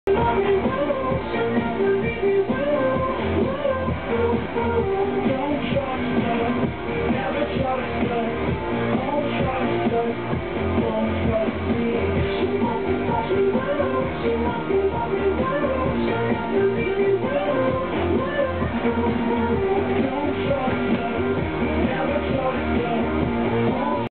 Kaylee & Morgan
Enjoying the Twisters at Billing Aquadrome